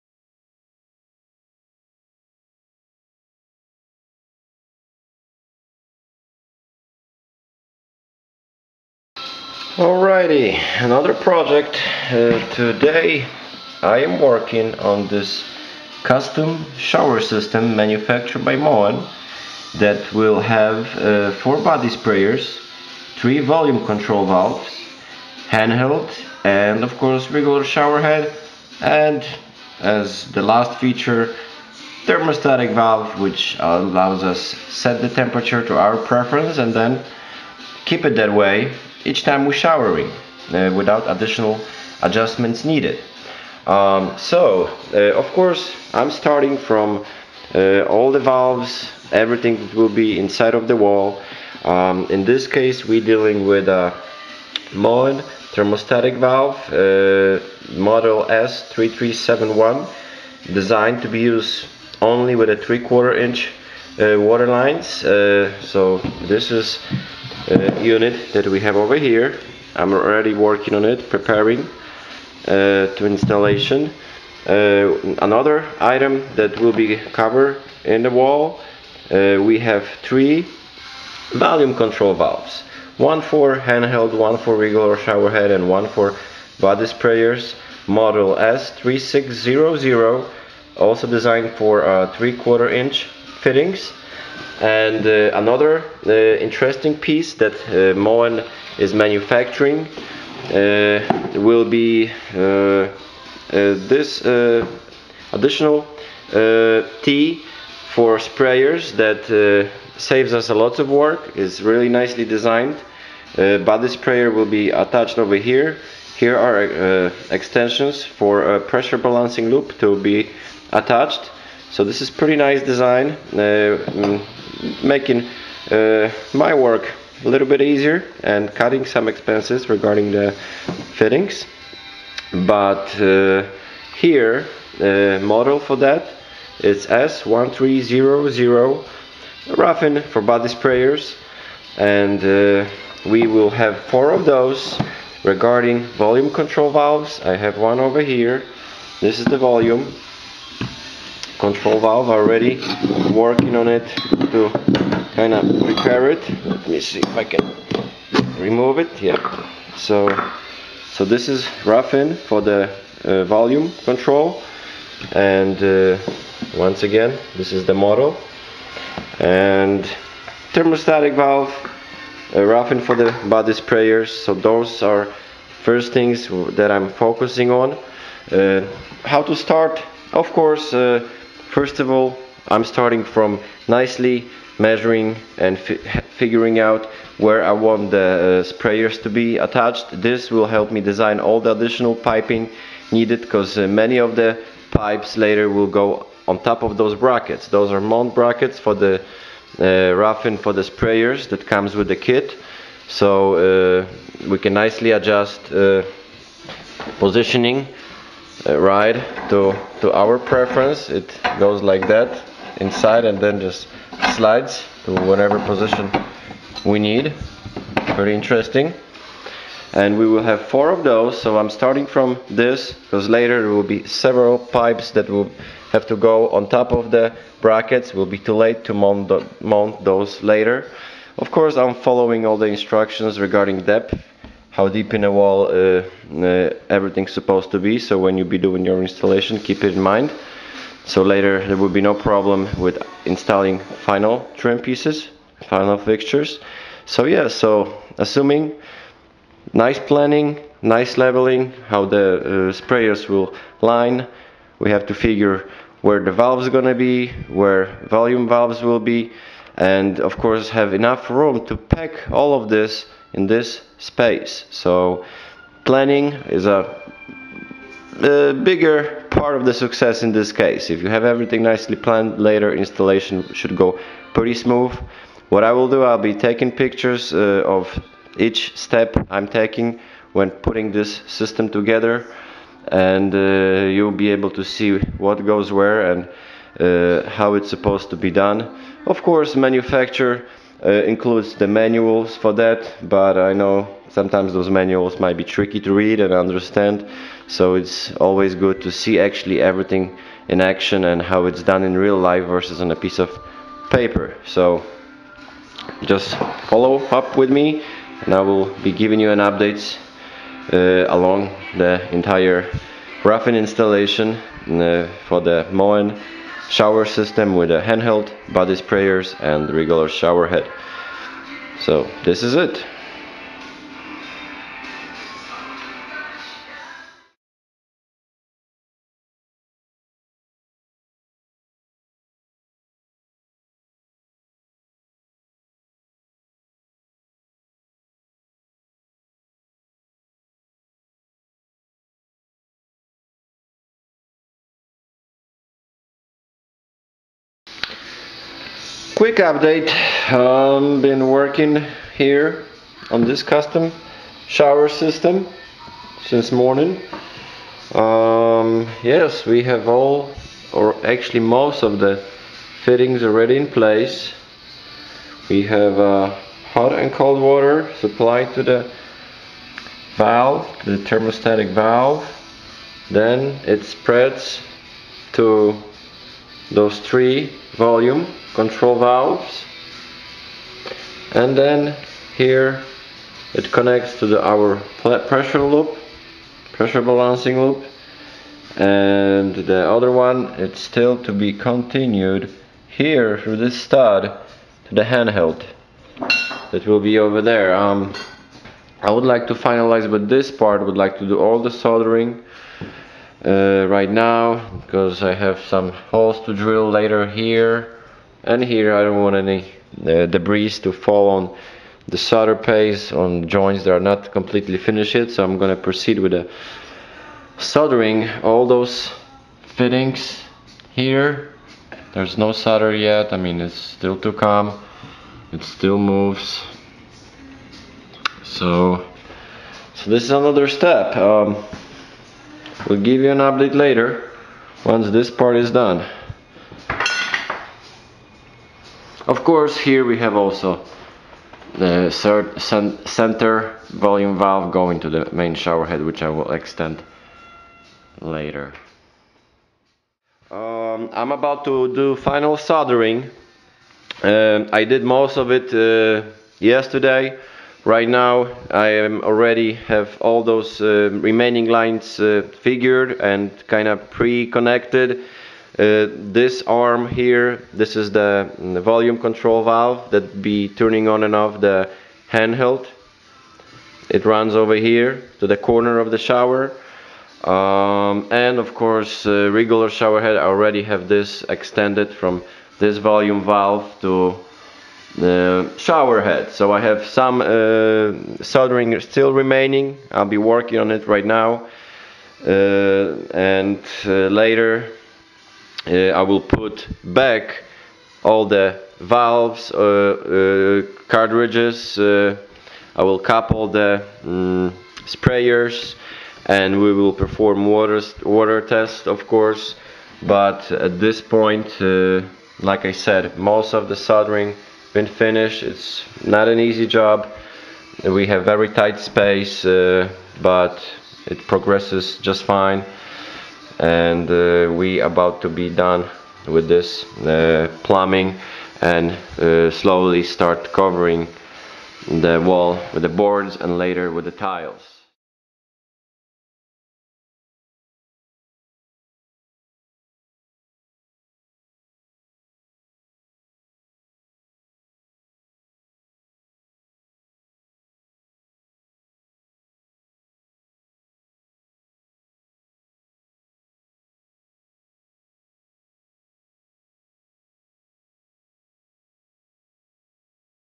(part-1) MOEN TS276 Custom vertical SPA shower system installation, sprayers, hand held 3/4" valve
I am presenting installation from scratch for MOEN custom SPA shower system Ts 276.
Preassure balancing loop, ¾" water lines, volume control Thermostatic valve installation, hand shower installation, custom Flush Mounted 4 Body Sprays,